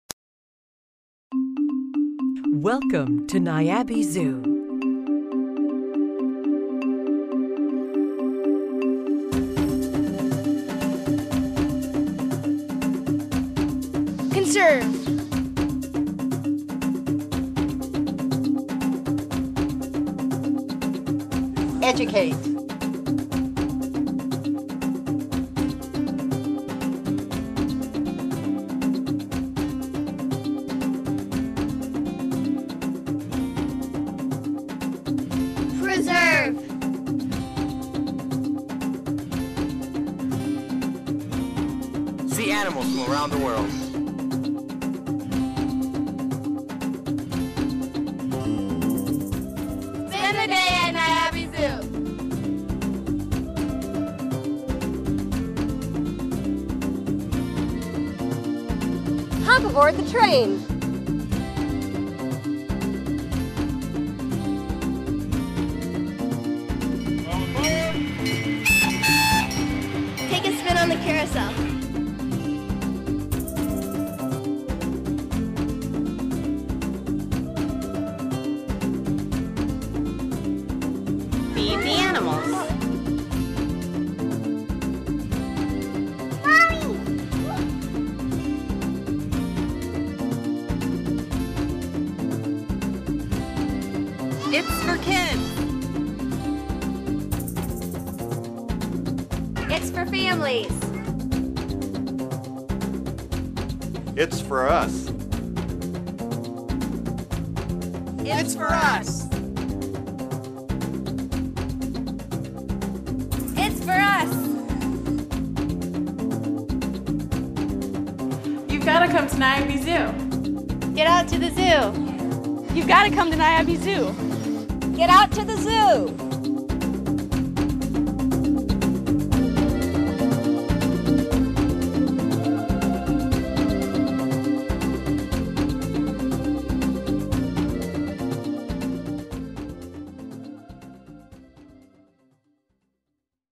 Niabi Zoo Promo Video
Take a tour of Niabi Zoo and see all of the animals and attractions this popular Quad City destination has to offer.